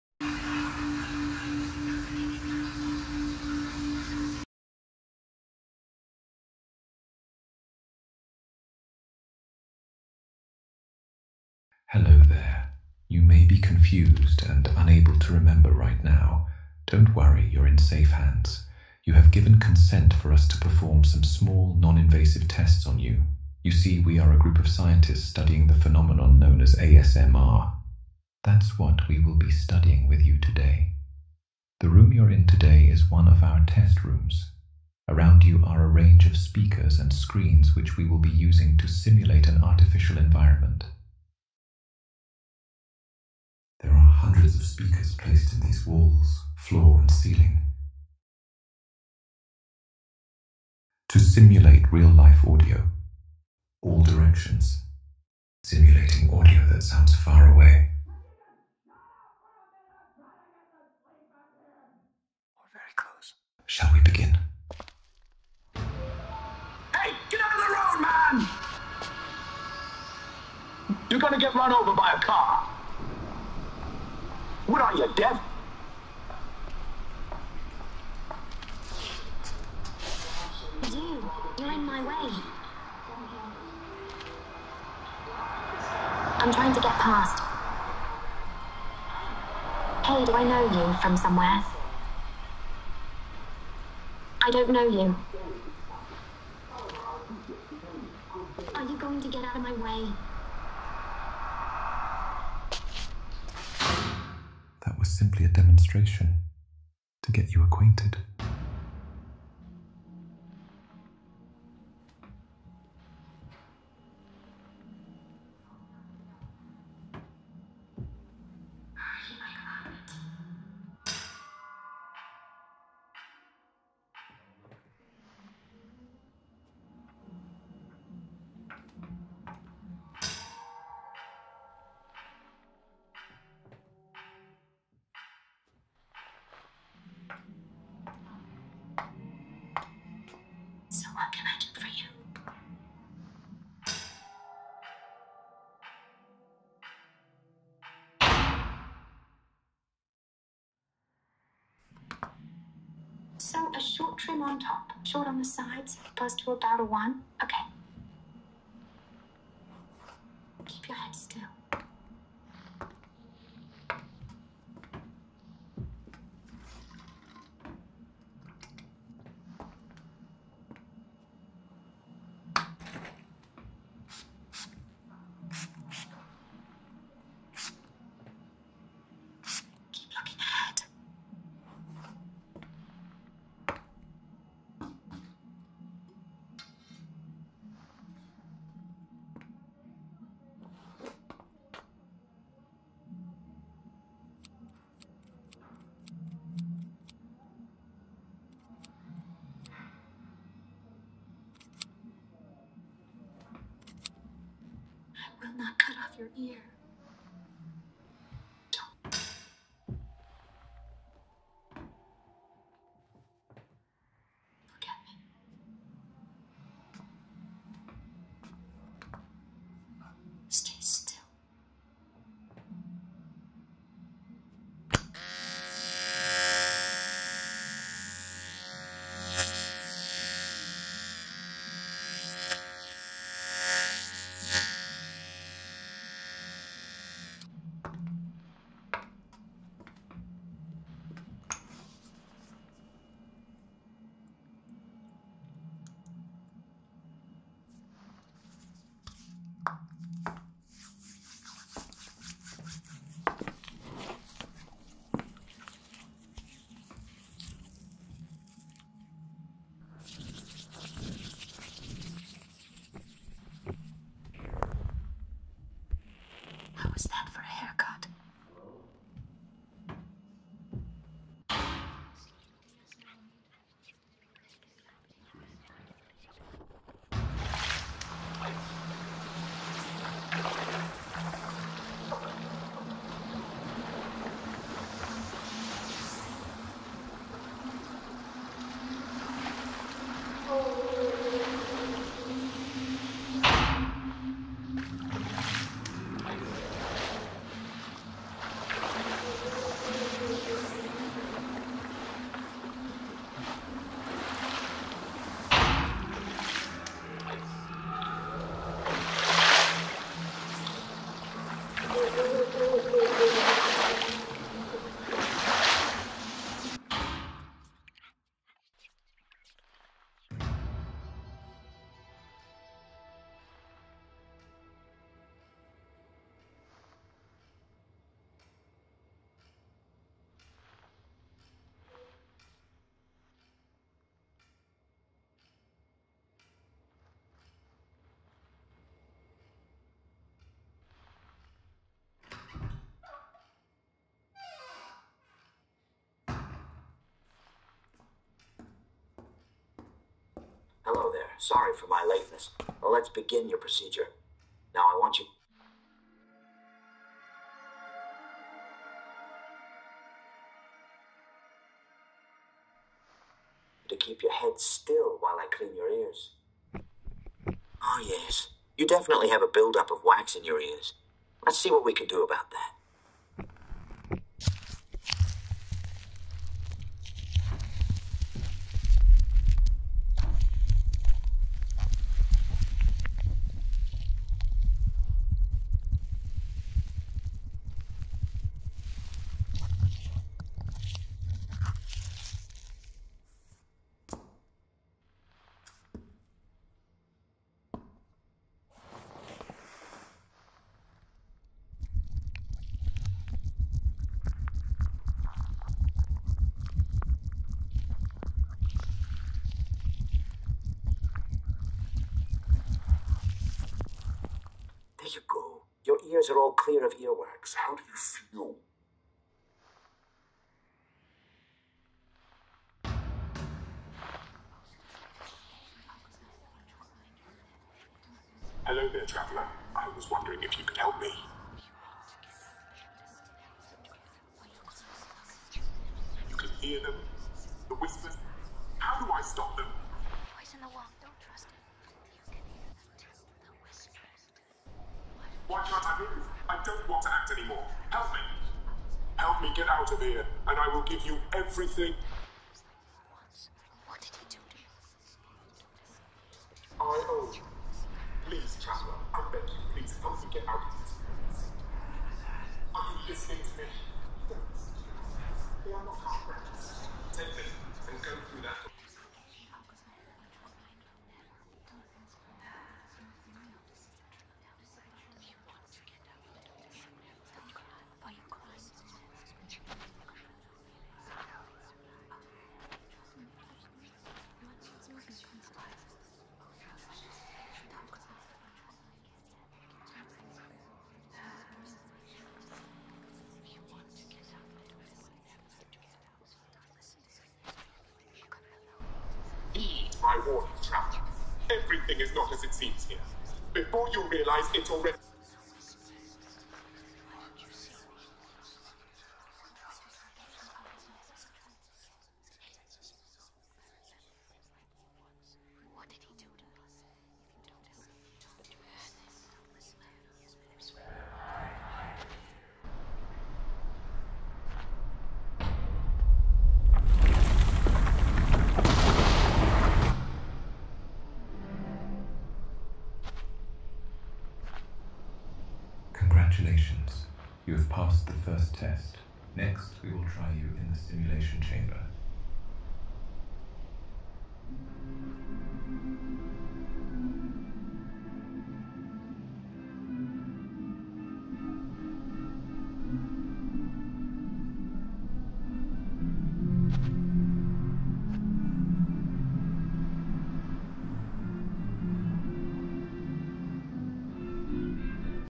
This room was built for ASMR (wear headphones)
Rules -
1. Wear Headphones
2. Don't Leave The Room

It's our specialty! Can you experience the phenonom known to the internet as ASMR? Take this short test and find out, if you experience any kind of sensation known as "tingling" you can get it. It's experience in the head or spine.